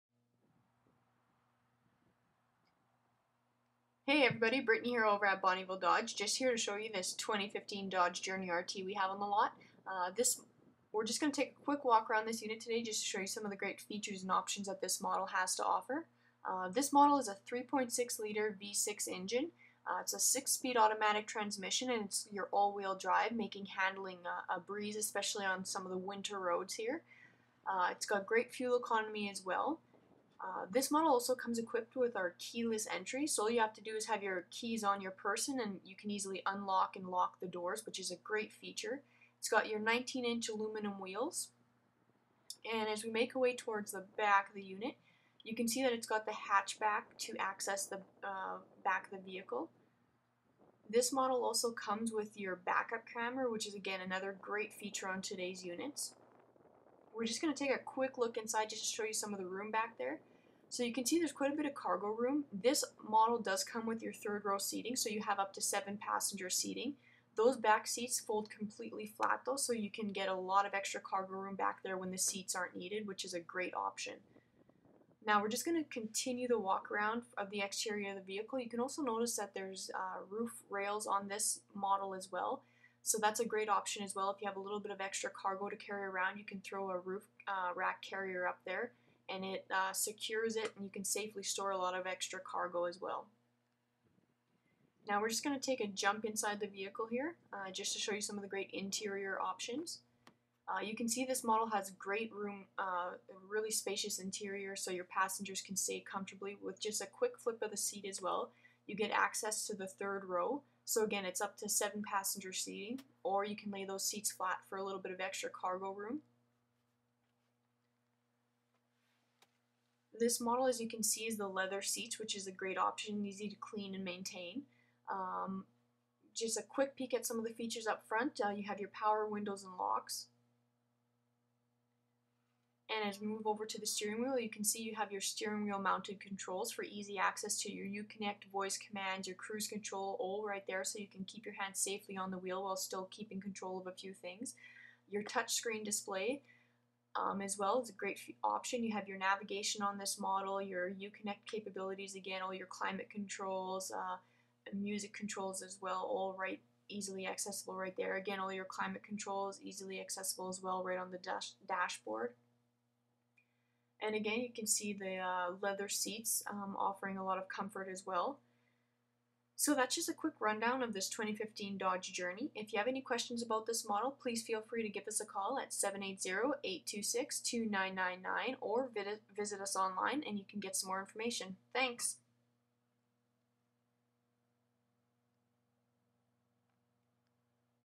2015 Dodge Journey R/T
The Dodge Journey is a great option if you are in the market for a new family friendly vehicle. As Canada’s most affordable mid size crossover you have the room to haul the whole family and also everyone’s bags and extra cargo at a price you can smile at. With the comfortable premium leather interior and added features like Uconnect, touchscreen display, the navigation and sound group this vehicle has just the right amount of features to keep you comfortable and content on your next driving adventure.